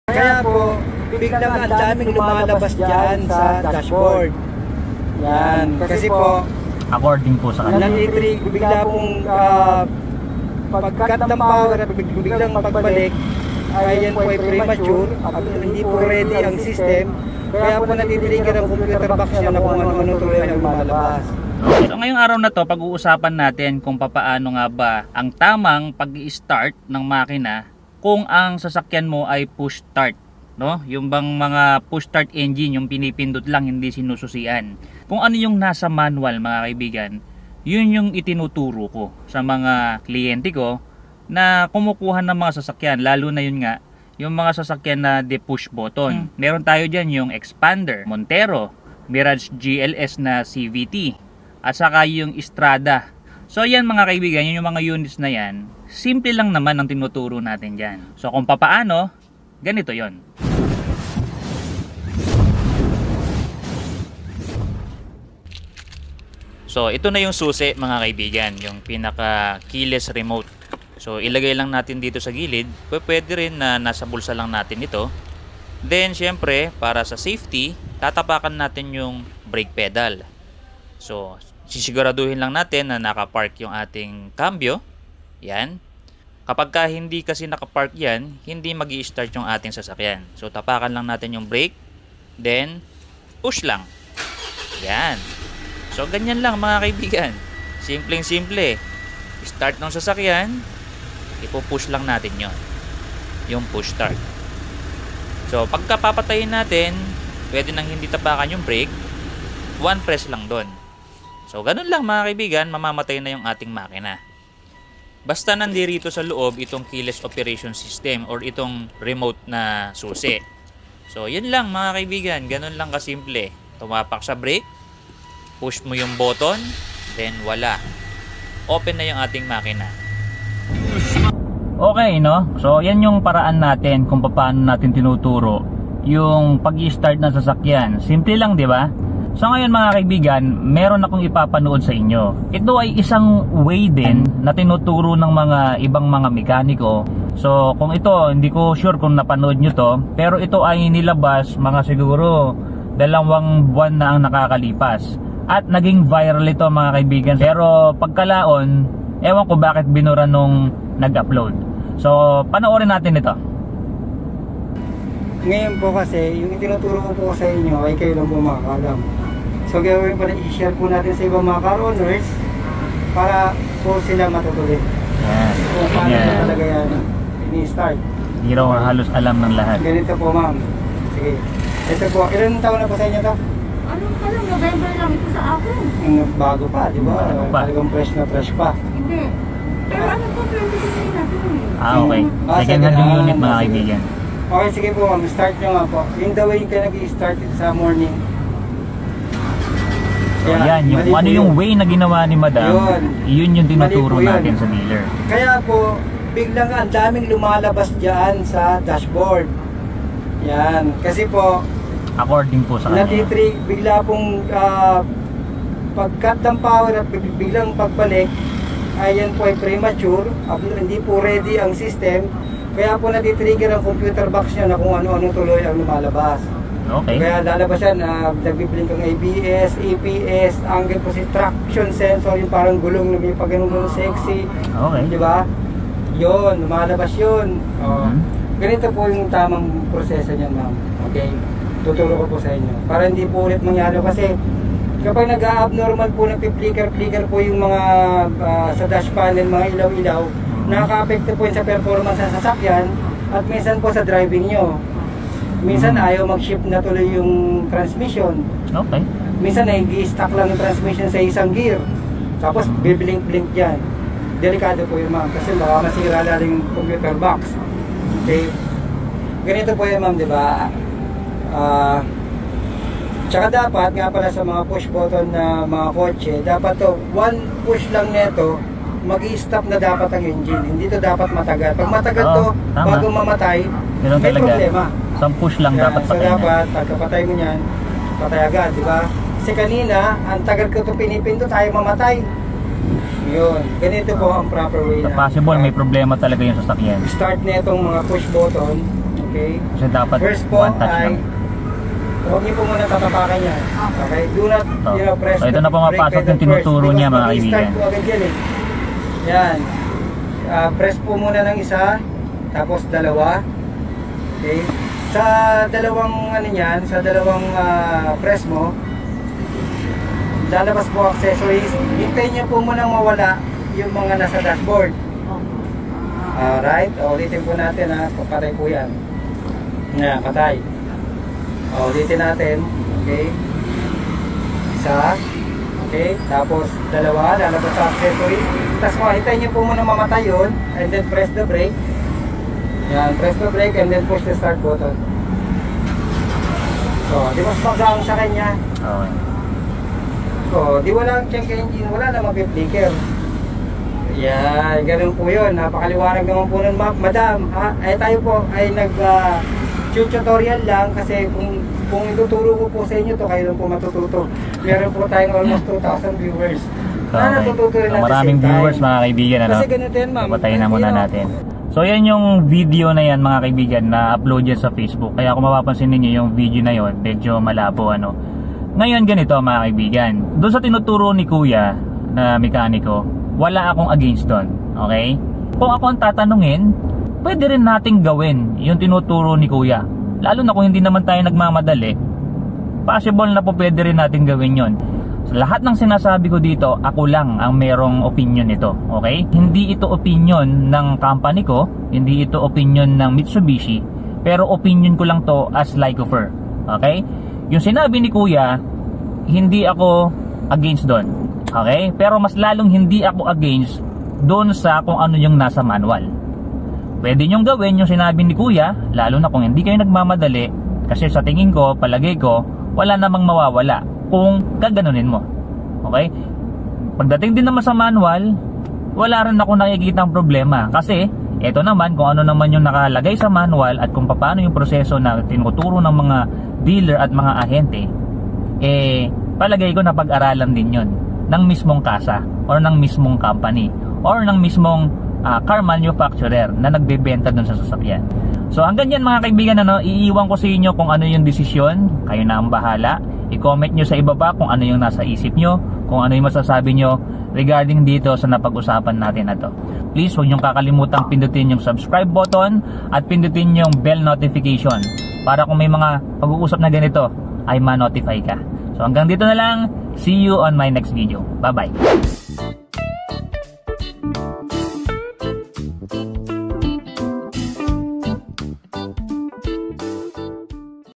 Car Engine Start / Stop push Button || Paano nga ba ang tamang Paraan?
Paano ka ba mag start ng Sasakyan mong may Engine Start /Stop Button? Dito sa Video na ito ipapakita ko kung paano.

Hi Im LYCOPHER DE LEON
Car Sales Representative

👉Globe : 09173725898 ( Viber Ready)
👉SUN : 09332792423

May TIKTOK din po ako. 🤪